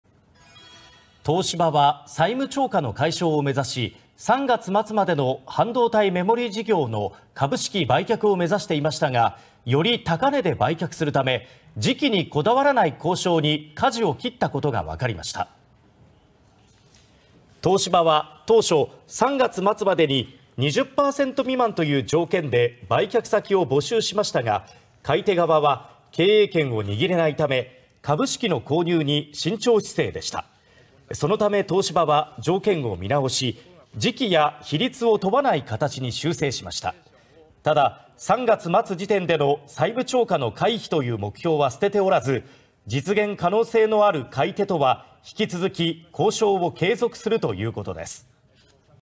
東芝の半導体事業売却 決定時期「4月以降」も(17/02/16)
東芝は、債務超過の解消を目指して来月末までの半導体メモリー事業の株式売却を目指していましたが、より高値で売却するため、時期にこだわらない交渉にかじを切ったことが分かりました。